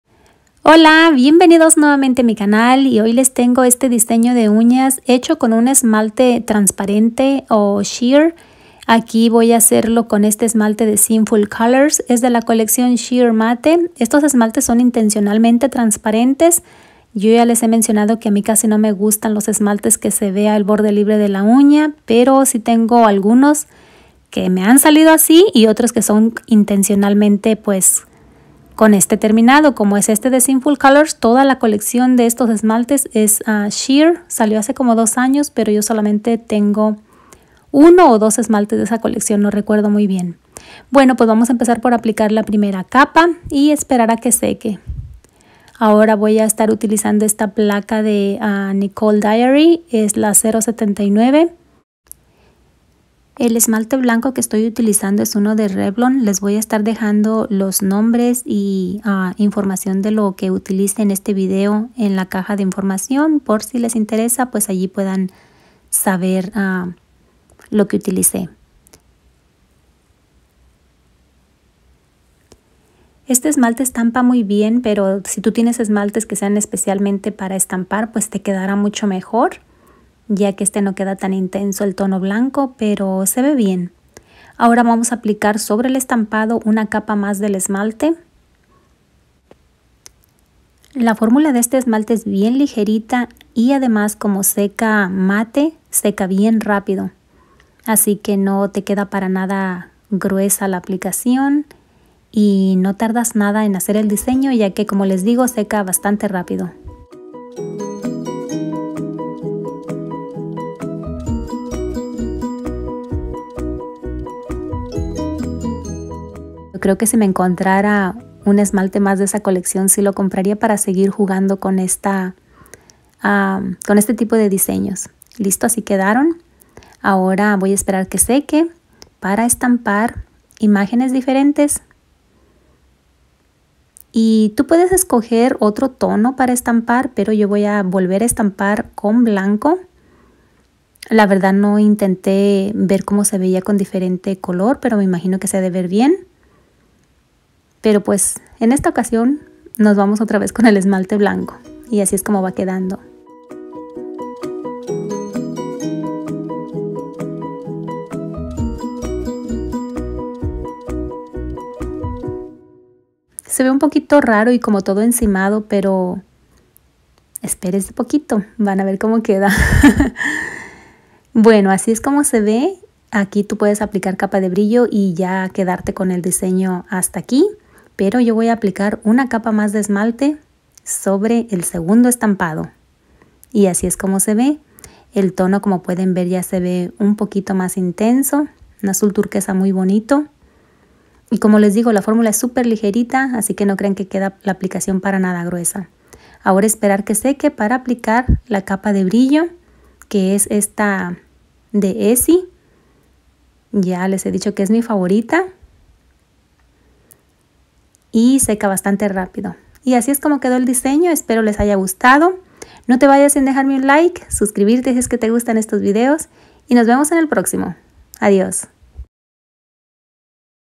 Diseño de Uñas Utilizando Esmalte Transparente | Swatches Y Mas
Hola!! Hoy les tengo un diseño de uñas facil y muy sencillo donde puedes darle uso a esmaltes trasparentes o sheers  este diseño lo vi hace mucho en Instagram y asta ahora me dio tiempo hacerlo aunque a mi no me quedo igual me gusto mucho el resultado 🥰

Productos Utilizandos
- SinfulColors Turnt-Quoise
- Revlon HD Early Bird
- Placa Nicole Diary -079
- Topcoat Essie Good To Go